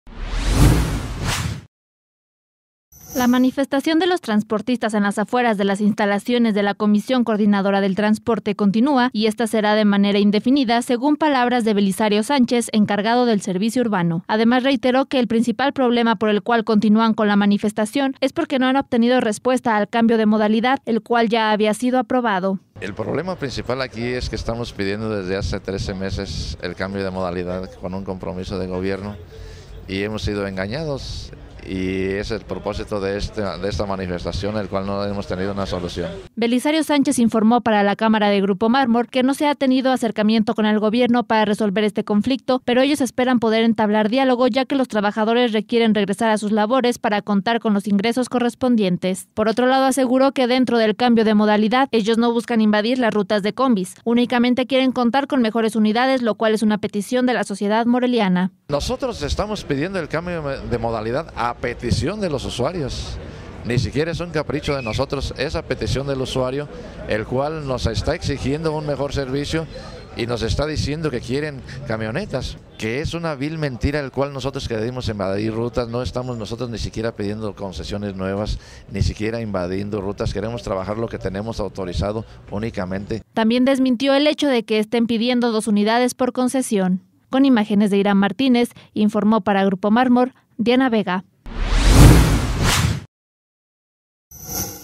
Continúa manifestación de transportistas en COCOTRA
La manifestación de los transportistas en las afueras de las instalaciones de la Comisión Coordinadora del Transporte continúa y ésta será de manera indefinida según palabras de Belisario Sánchez, encargado del servicio urbano.